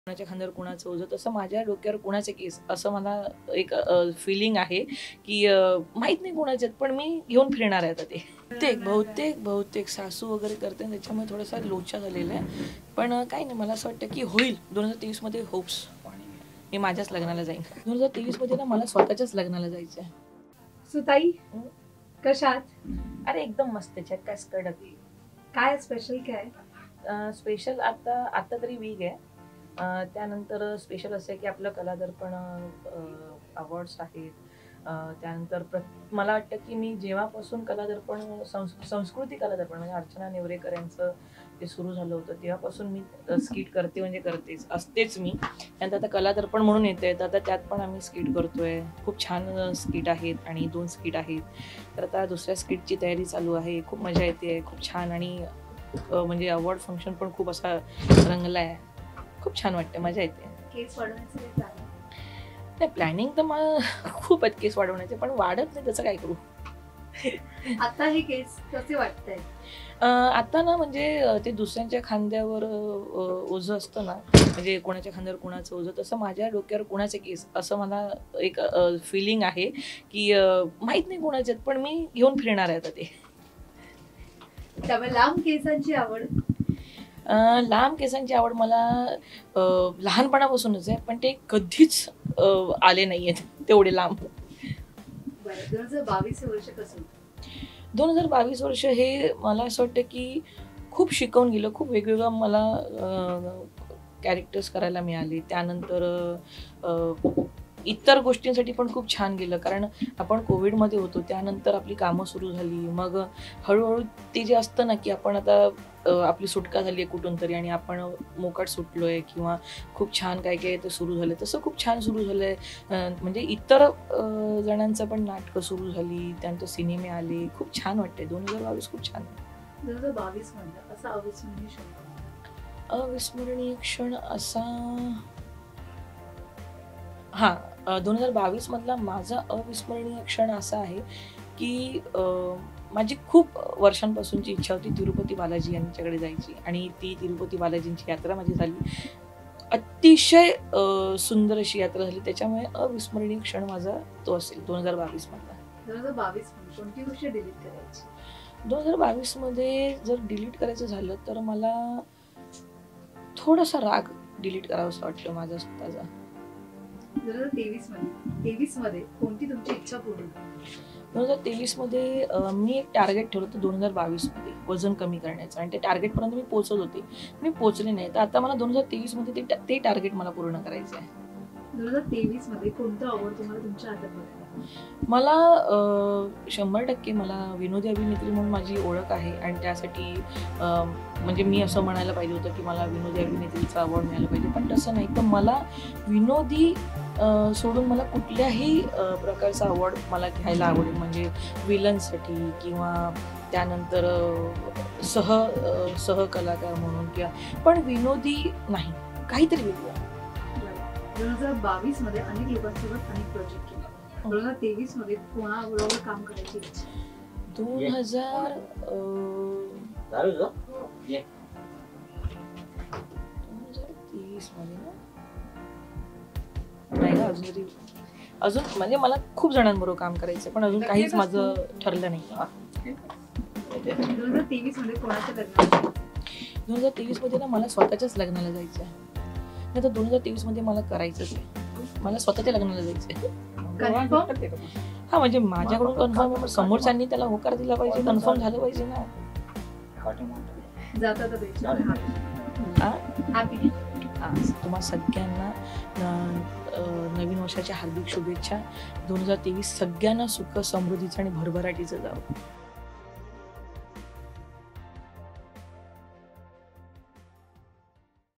Aarti Solanki || आरती सोलंकी लवकरचं अडणार लग्न बंधनात || Actor Updates || Hunch
आरतीसोबत रंगला Rapid Fire Round